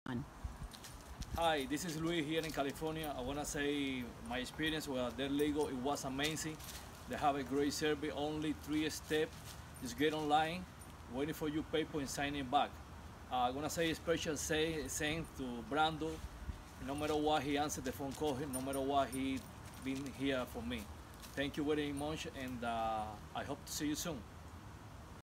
Dirt Legal Made My Military Humvee Street Legal
Want to register your military vehicle? Dirt Legal can make it happen. Hummers, LMTVs, CUCVs, converted exploration vehicles, even military trailers - you name it, we can make it street legal.